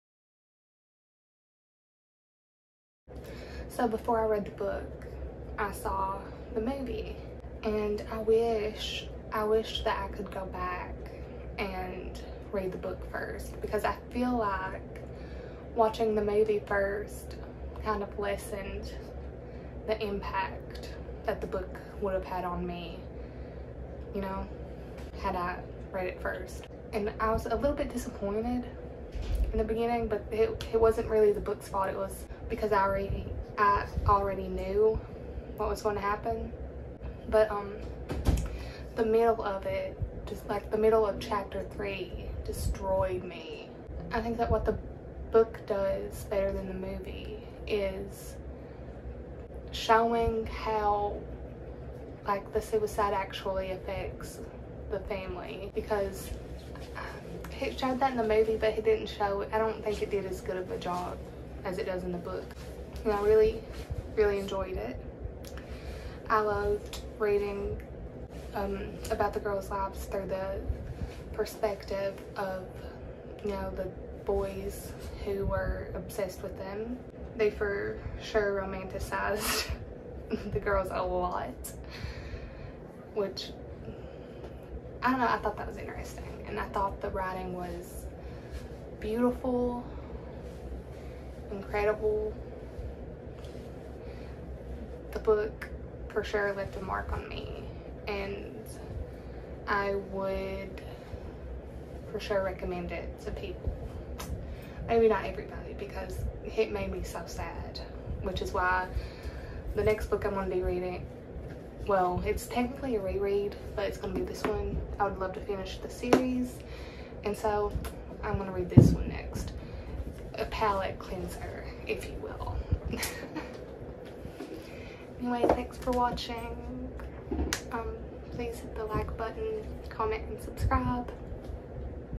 Book Review | The Virgin Suicides by Jeffrey Eugenides
My first book review on YouTube! I hope you enjoy it!! 💖💖💖